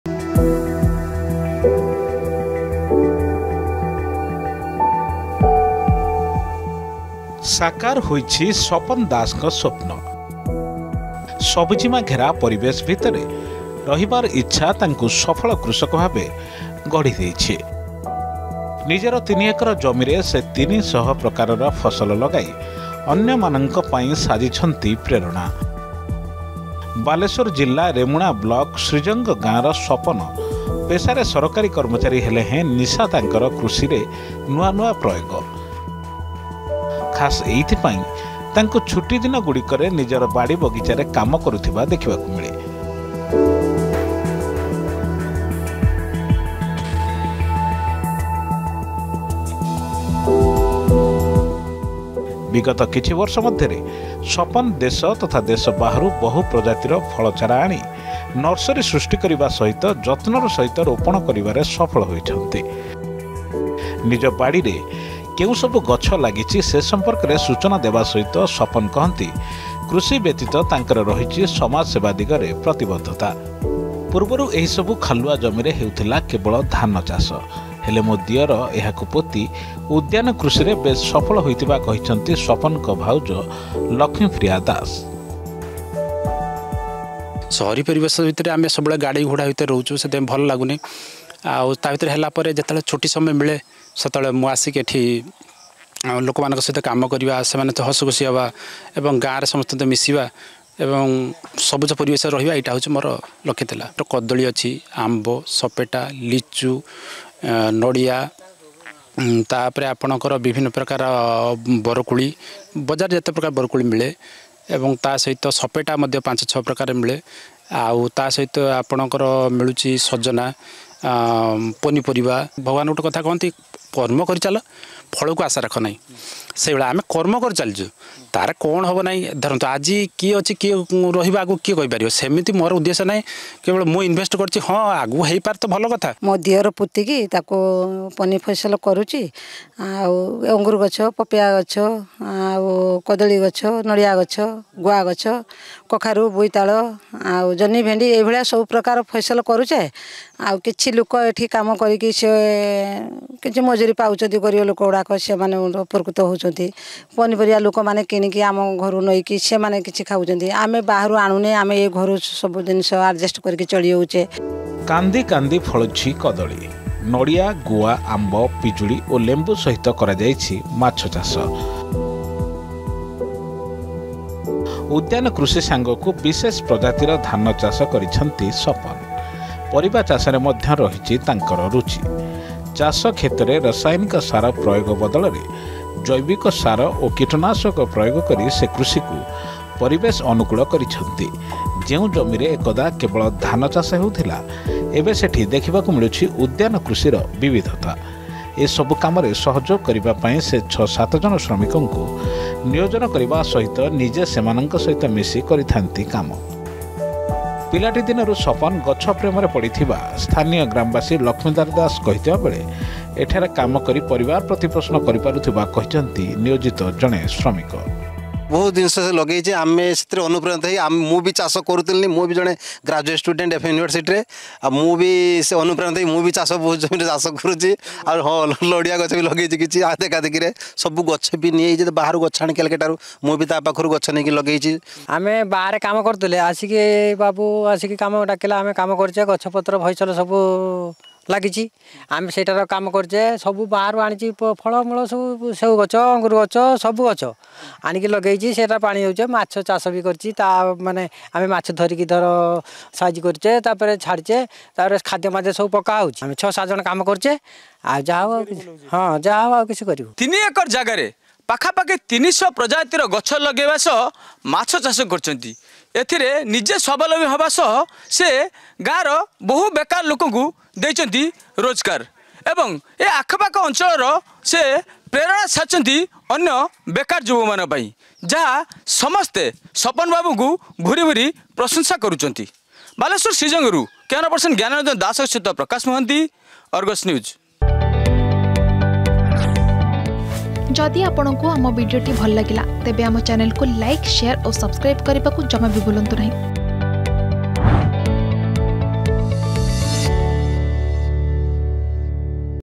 ସଫଳ ଚାଷୀ ସପନ | Good News
୩ ଏକର ଜମିରେ କରିଛନ୍ତି ୩୦୦ ପ୍ରଜାତିର ଫସଲ । ପେଶା ସରକାରୀ ଚାକିରୀ ହେଲେ ନିଶା ତାଙ୍କର କୃଷି । ସବୁଜିମାମୟ ପରିବେଶ ଭିତରେ ରହିବାର ଇଛା ନେଇ ବାଲେଶ୍ୱର ଜିଲ୍ଲା ରେମୁଣା ବ୍ଲକ ଶ୍ରୀଜଙ୍ଗ ଗାଁର ସପନ ଦାସ ଦେଖି ଆସିଛନ୍ତି ସ୍ବପ୍ନ ।